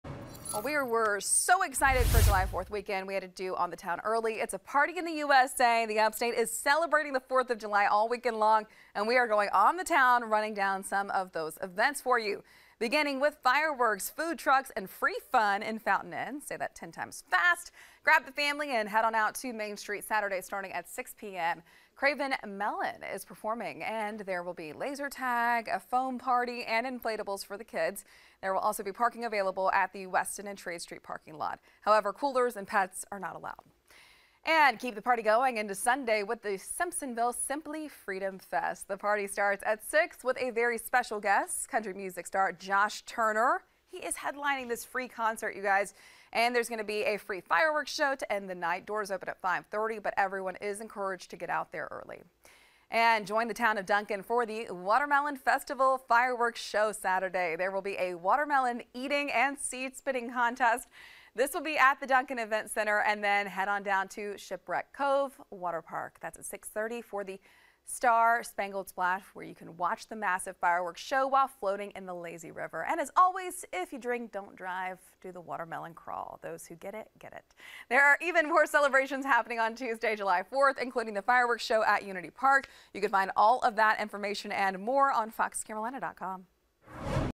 On the Town: 6/28
Highlighting what's happening in the area including Fountain Inn Spectacular, Simpsonville Freedom Fest and Watermelon Festival.